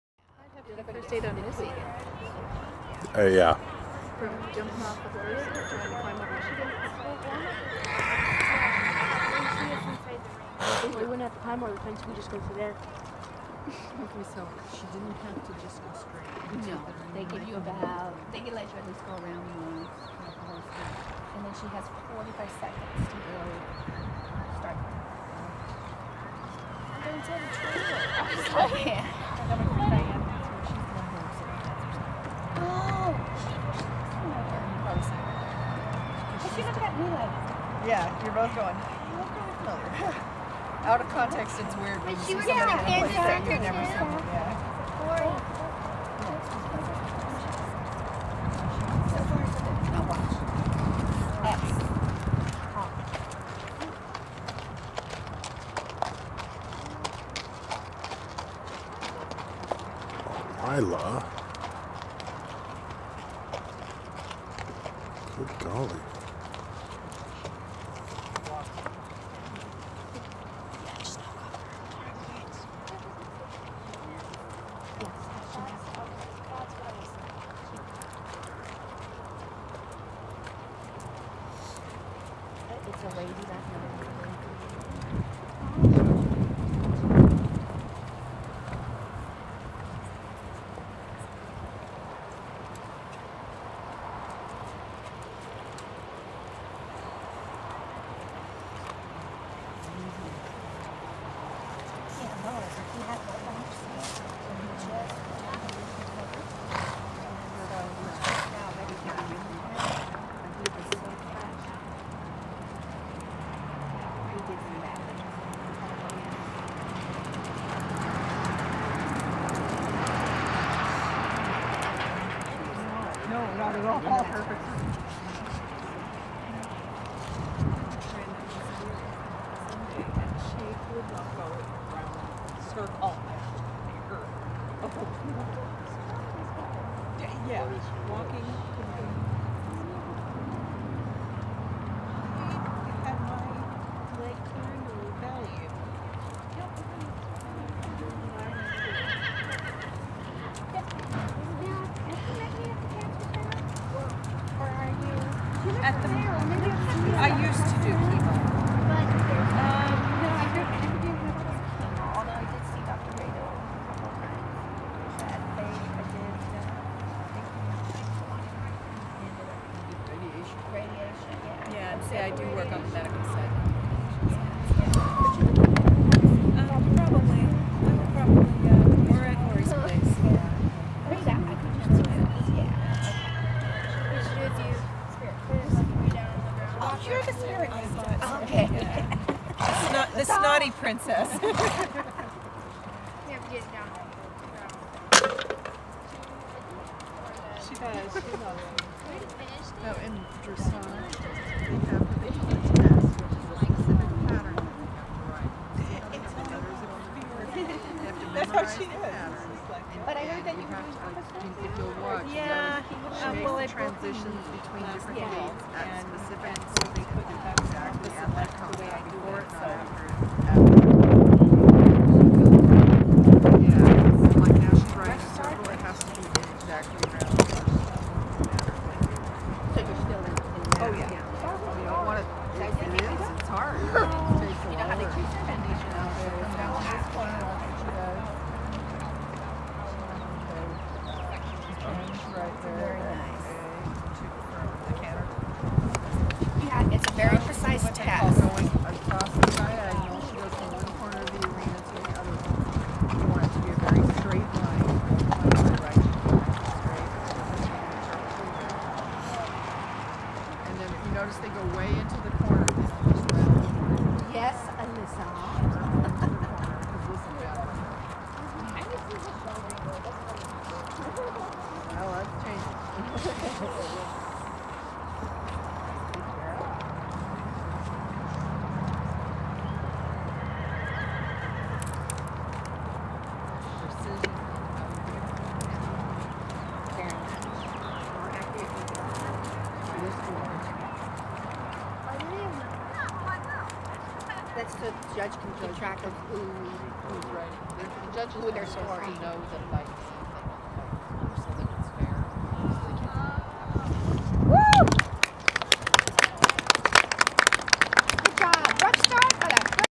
Training, Test 4 = RRC Show 4-24-10
Missy and Lyla performing dressage test.